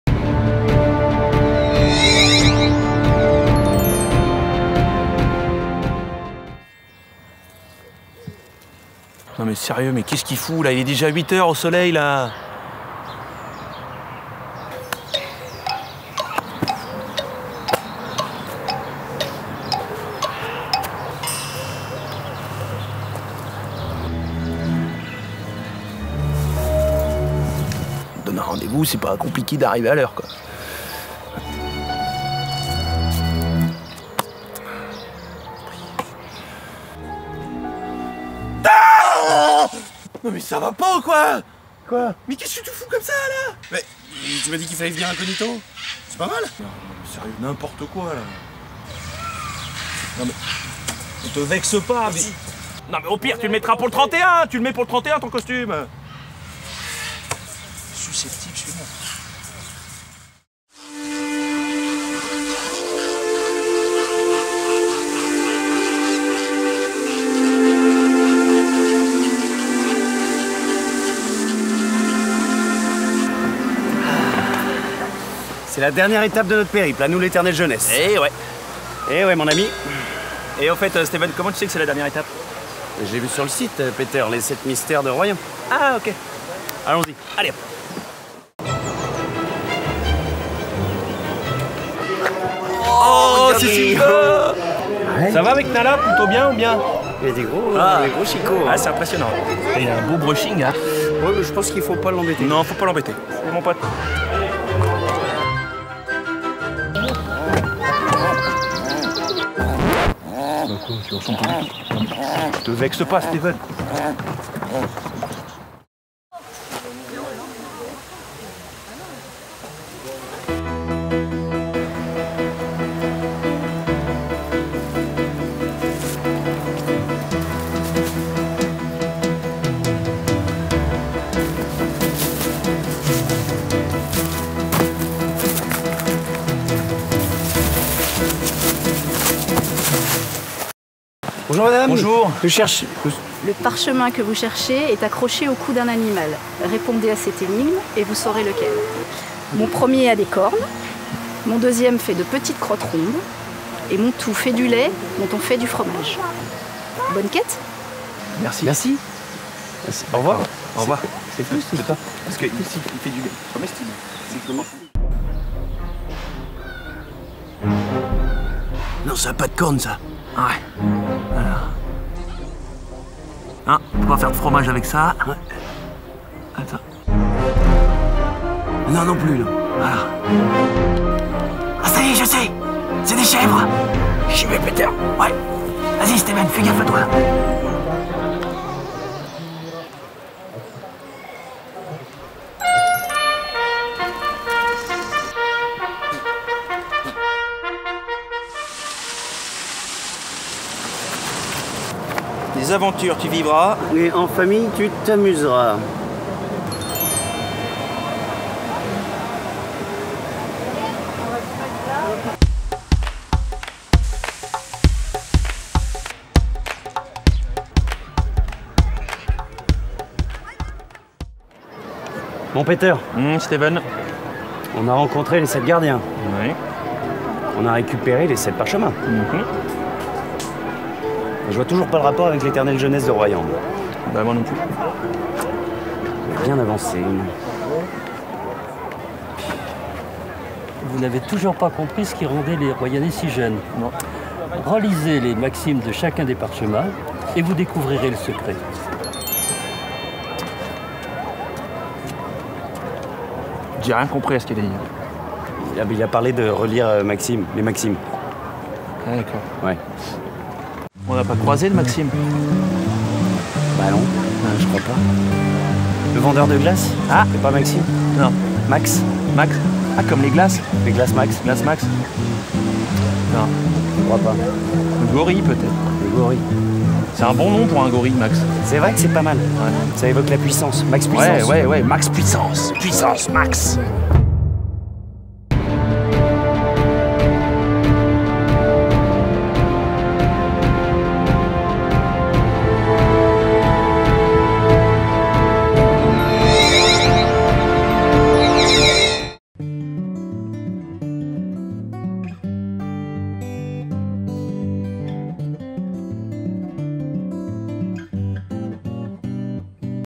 Jeu des 7 mystères de Royan - Episode 7 - Zoo La Palmyre
Nos 2 aventuriers terminent leur course aux 7 mystères de Royan au Zoo de la Palmyre. Vont-ils enfin découvrir le secret de l'éternelle jeunesse de Royan ?
Une surprise vous attend en fin d'épisode !